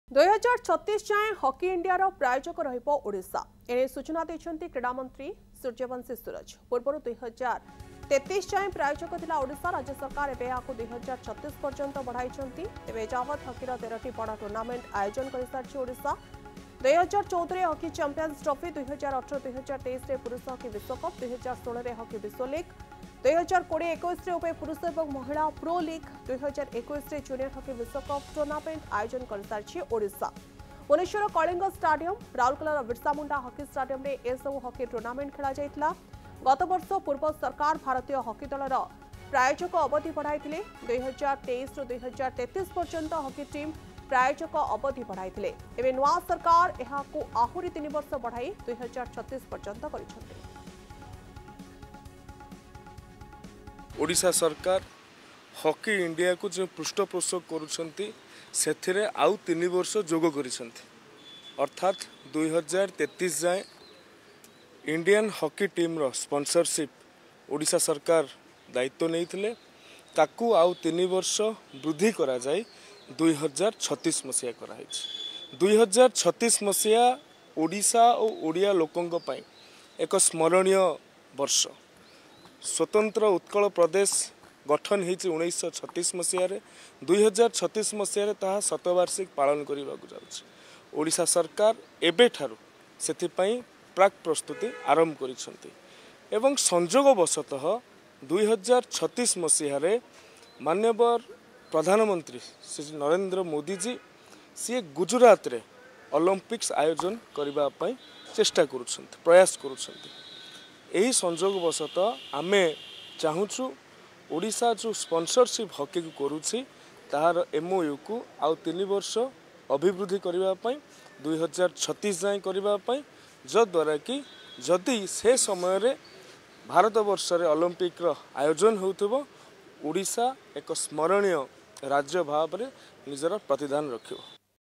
Sports Minister Suryabanshi Suraj Extends Sponsorship Of Indian Hockey Till 2036
Sports Minister Suryabanshi Suraj has announced the extension of sponsorship for Indian Hockey until 2036. This initiative aims to bolster the sport's development and support across the nation. Discover more about this significant commitment to Indian hockey.